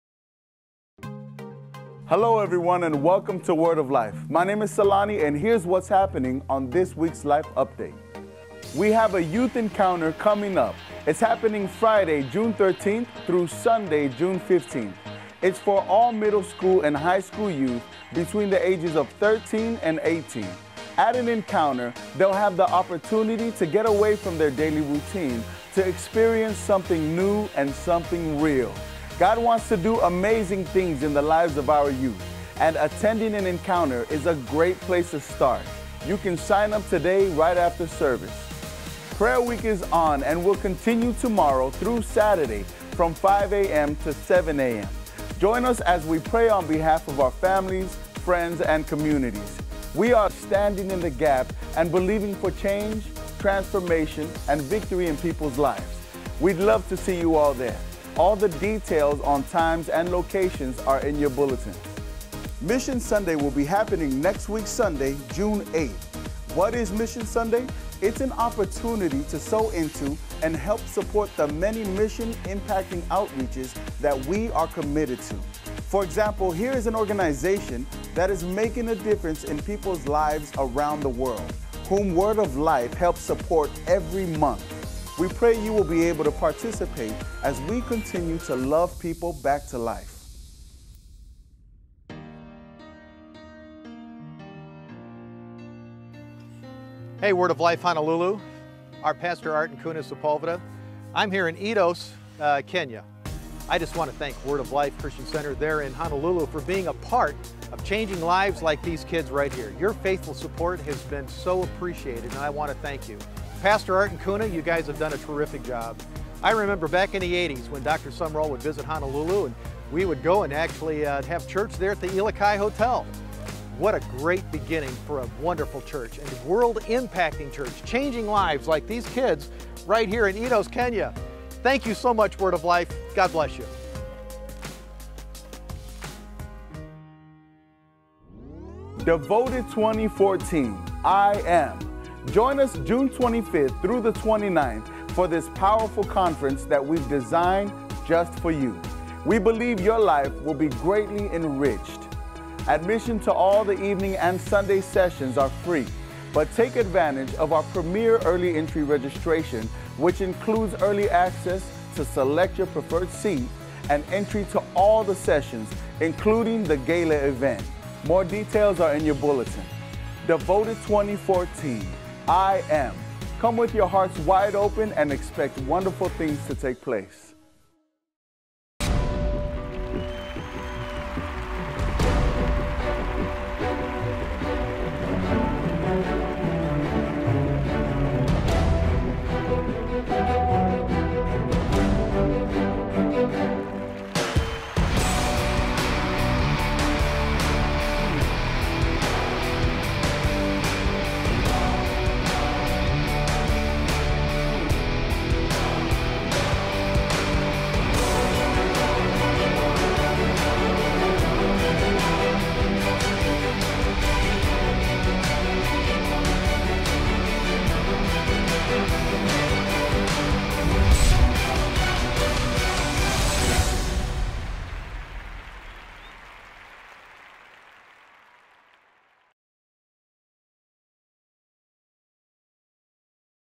Life Update for Sunday, June 1, 2014
Here's our Life Update for the week of Sunday, June 1, 2014! Take a look at what's happening at Word of Life...

- Youth Encounter (June 13-15)
- Prayer Week (June 1-7)
- Mission Sunday (June 8)
- Devoted Women's Conference 2014 (June 25-29)

For more information on our church, Word of Life Christian Center, in Honolulu, Hawaii, you can visit our